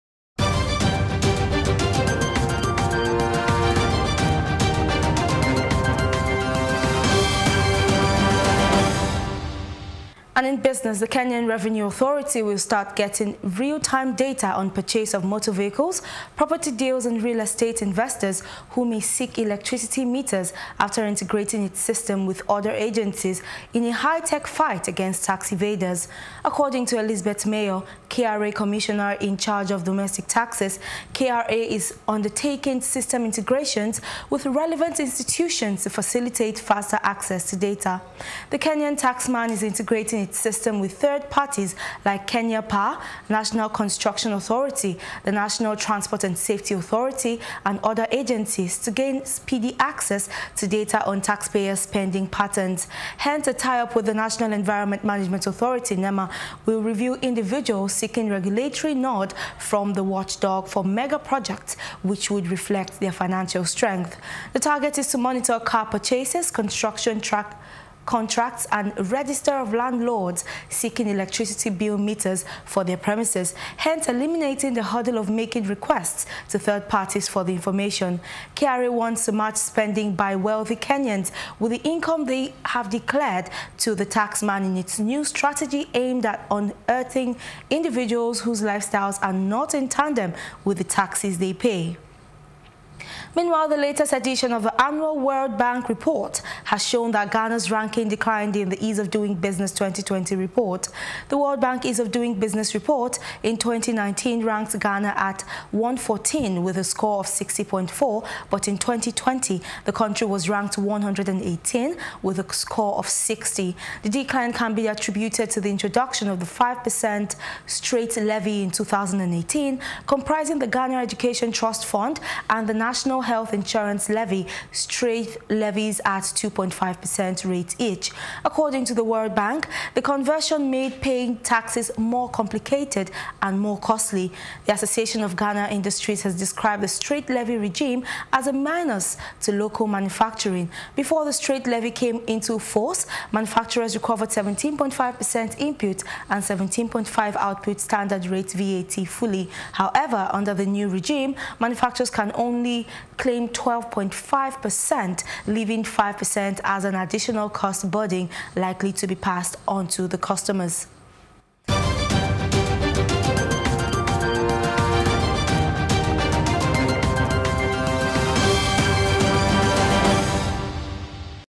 Kenyan Revenue Authority to Go After Tax Evaders
The Kenya Revenue Authority will start getting real-time data on the purchase of motor vehicles, property deals and real estate investors who may seek electricity meters after integrating its system with other agencies in a high-tech fight against tax evaders.